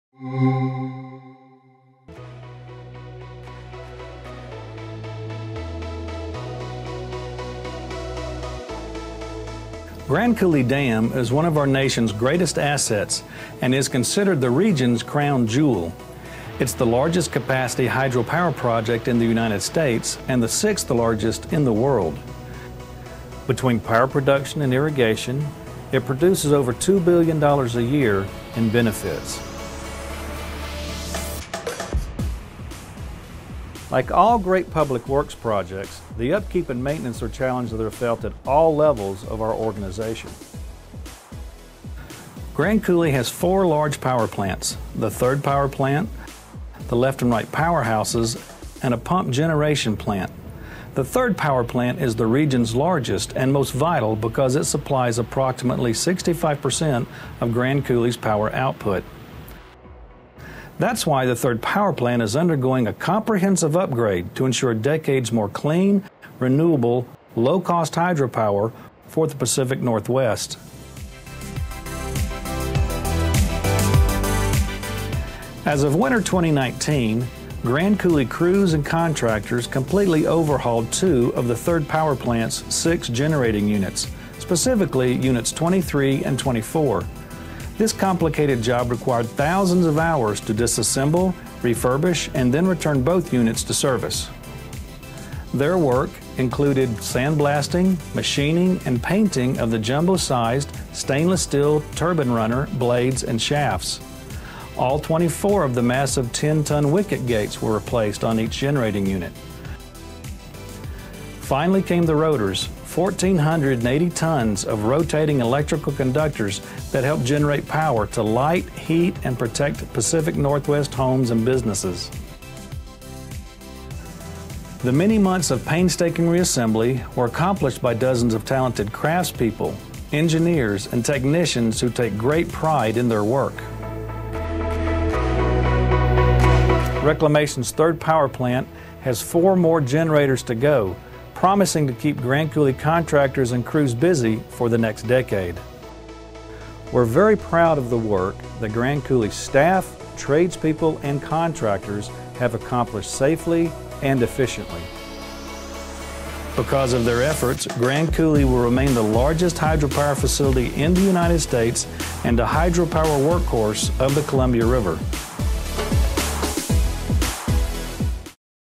Fit for the Future - Grand Coulee Dam's Third Power Plant Upgrades
Grand Coulee Dam‘s Third Power Plant is undergoing a comprehensive upgrade to ensure continued clean, renewable, low-cost hydropower for the future. Recently, Grand Coulee crews and contractors completely overhauled two of the Third Power Plant's six generating units; specifically, units 23 and 24. Grand Coulee is one of our Nation’s greatest assets and is considered to be the region’s crown jewel. It’s the largest capacity hydropower project in the United States and the 6th largest in the world.